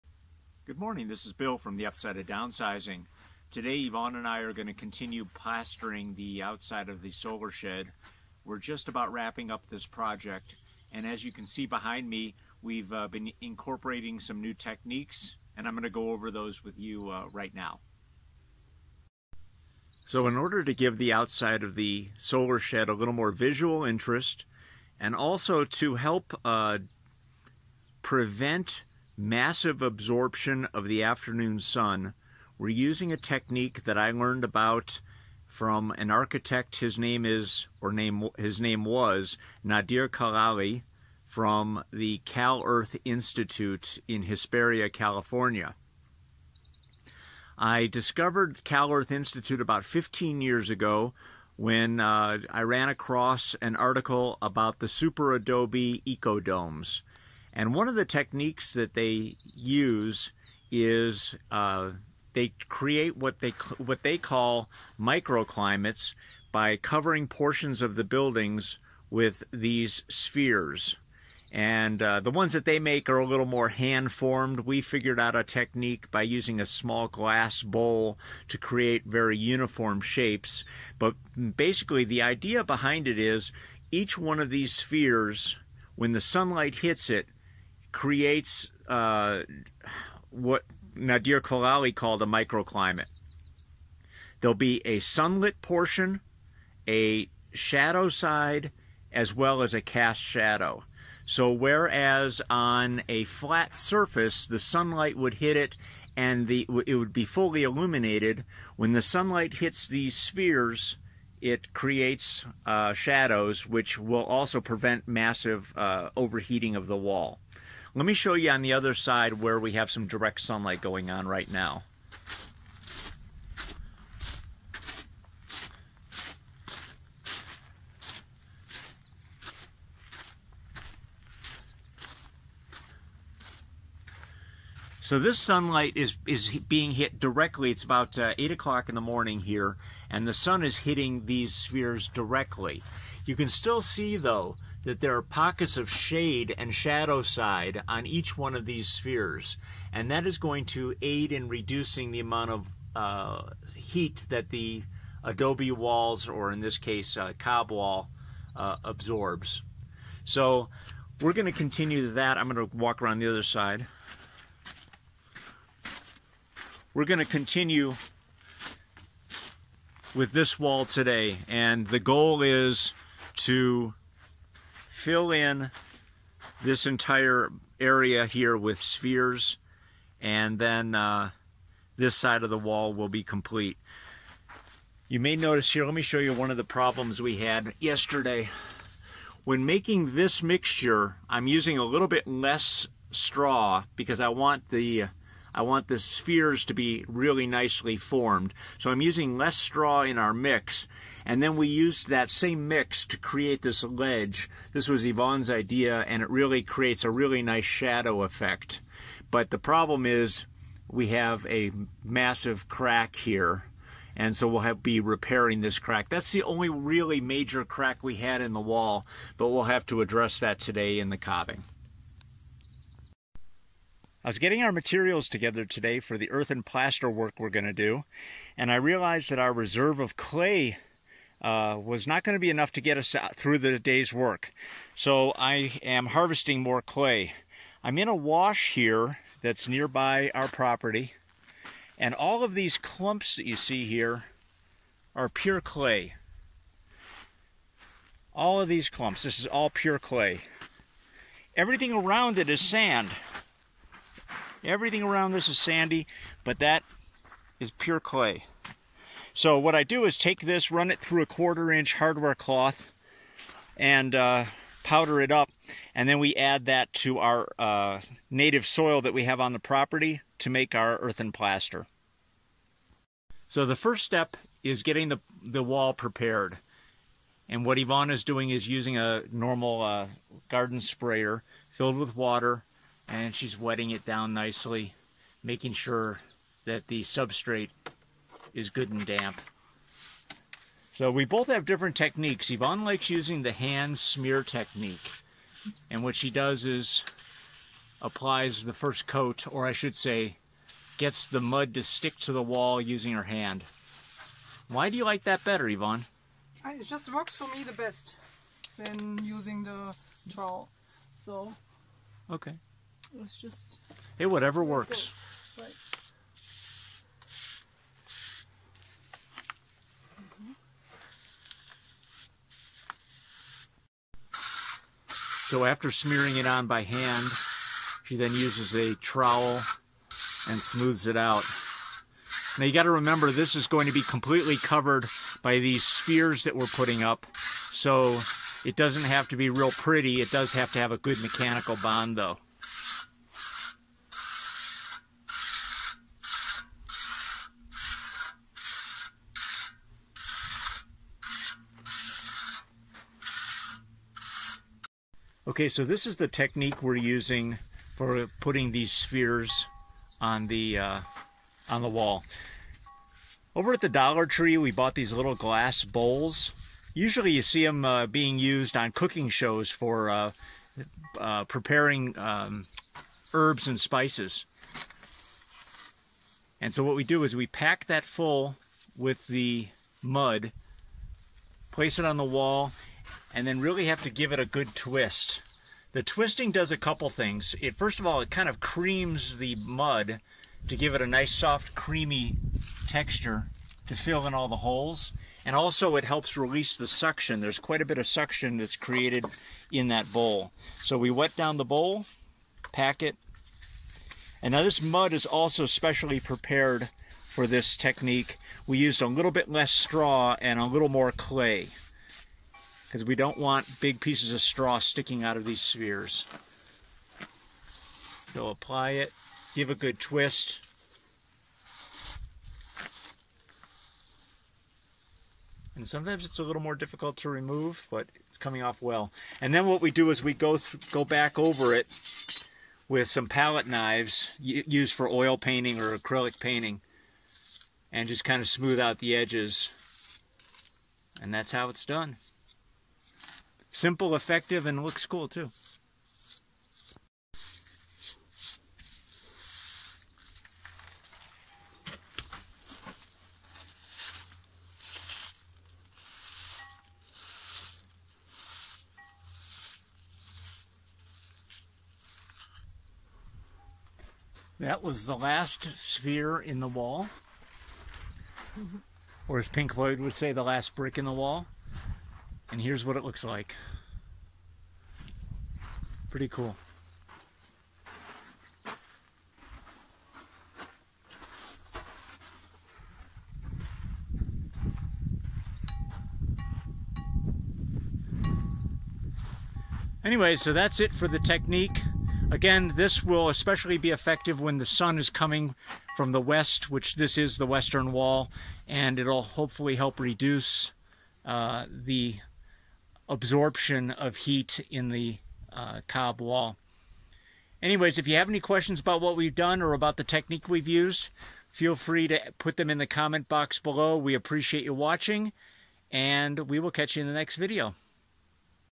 How To Reduce Heat Absorption on the Solar Shed!
in this video we are incorporating the technique we learn from Cal Earth Institute. we are creating "Cobb Spheres"  to help reduce the amount of direct impact from the afternoon sun.